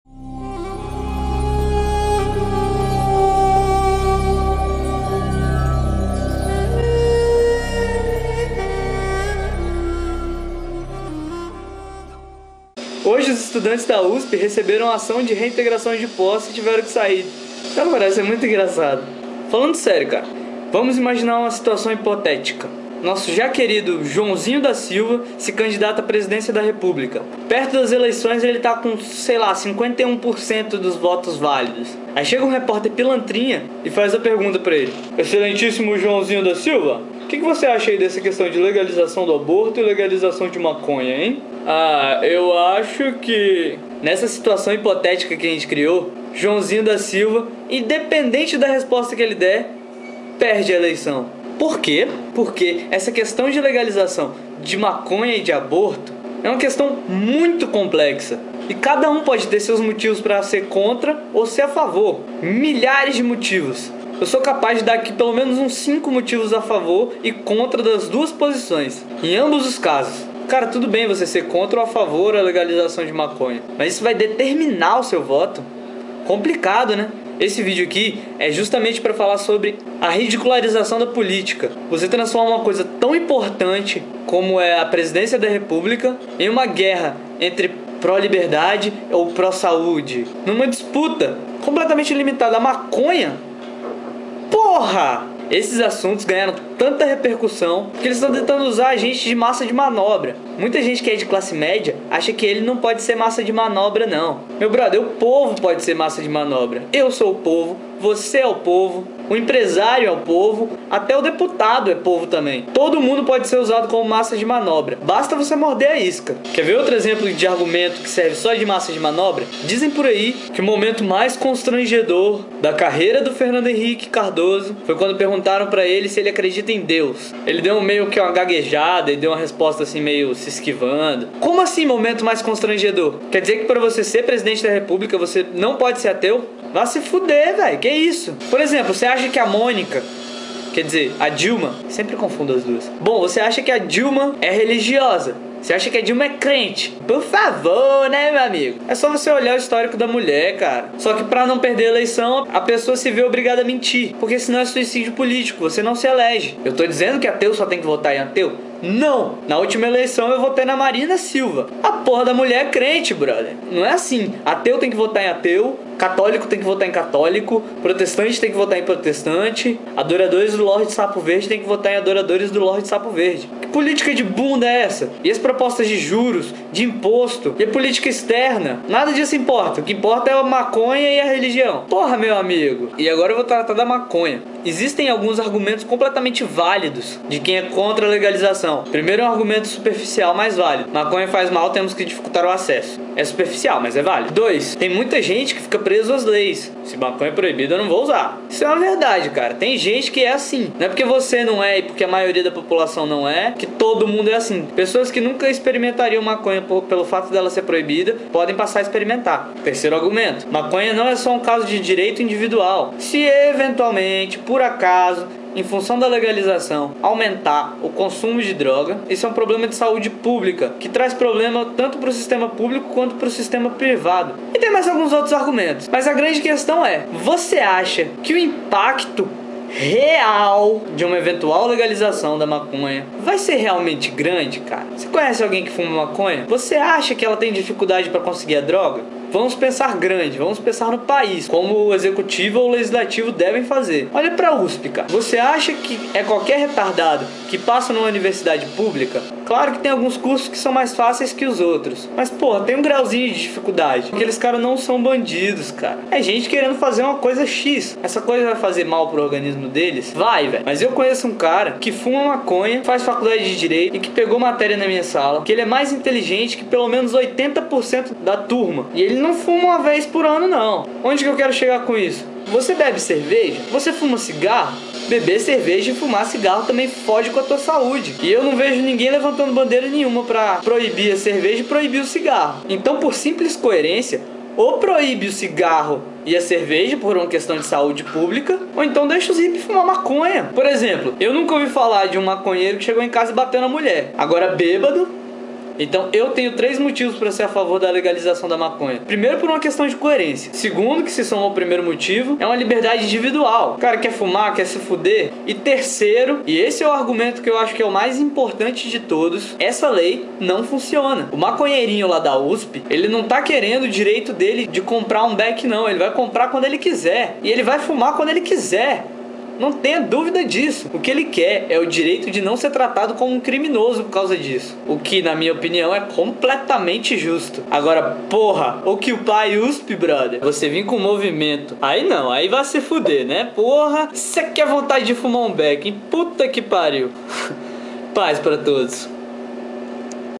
Ateu Informa: Occupy USP? Maconha e Aborto - Importância Desproporcional.
É óbvio que essa dúvida ia surgir. Não, eu não fumo maconha. Já experimentei porque eu era um playboyzinho de merda que queria tirar onda com os brother. Desde os 17 eu nunca mais fumei e experimentei com 16.

 Os caras tão lutando e apanhando para se posicionar contra a pilantragem por trás do mercado especulativo fora do país. Aqui dentro tão lutando e apanhando pelo direito de usar drogas.
Pra mim isso é, no mínimo, não saber regular prioridades.
Tudo bem que eles querem o direito de não ser considerados criminosos. Dá pra entender. Só que não deixa de ser cômico tanto engajamento por causa de drogas. E outra coisa, a depredação pública é injustificável

E outra coisa. É fato que:

- Existe gente que quer que os militares saiam por que a pura presença de policiais em um ambiente escolar incomoda.

- Existe gente que quer que os militares saiam para poder fumar no local sem precisar ficar com medo de tomar um bacu. Por que quem já tomou um bacu sabe que tem muito militar filho da puta que desce a porrada de graça.